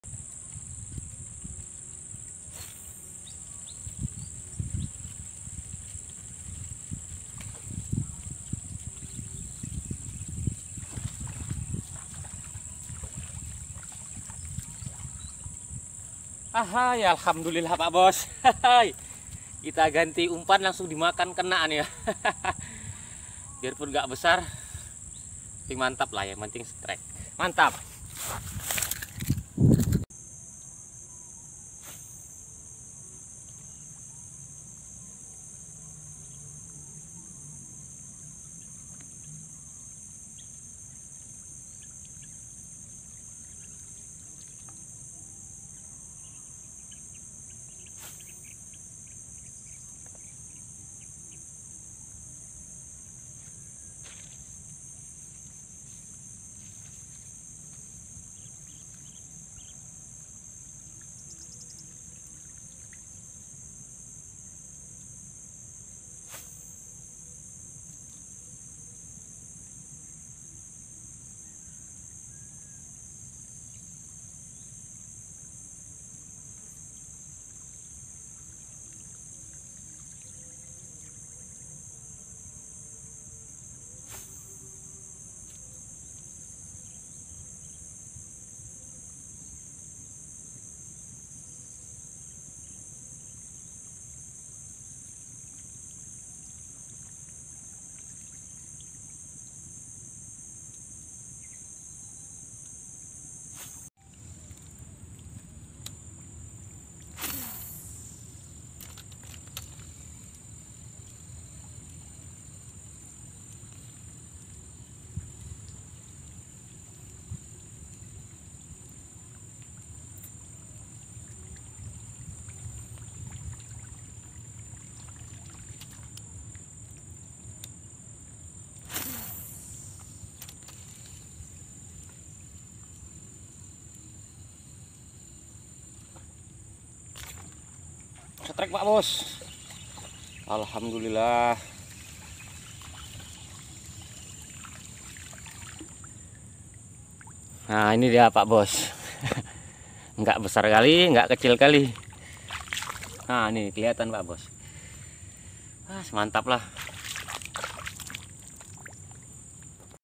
tes benang pe daido ancor 0,6 langsung strike
assalamualaikum saudara ku , hari ini saya mau tes kekuatan benang pe daido ancor 0,6